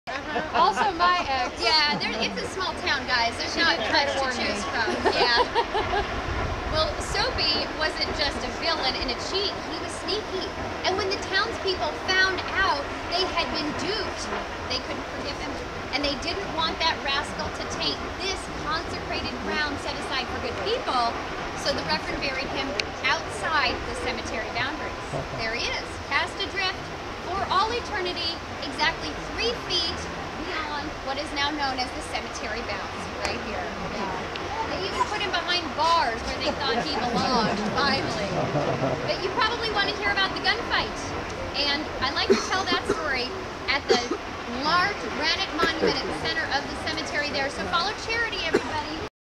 Colorful Skagway Winter Population 800 hardy souls Soapy Smith's Grave...
The Bold, Bad and the Ugly. Video 1. The story of the Skagway town Bad Guy Soapy Smith buried 3 feet outside the cemetery in shame. See Link...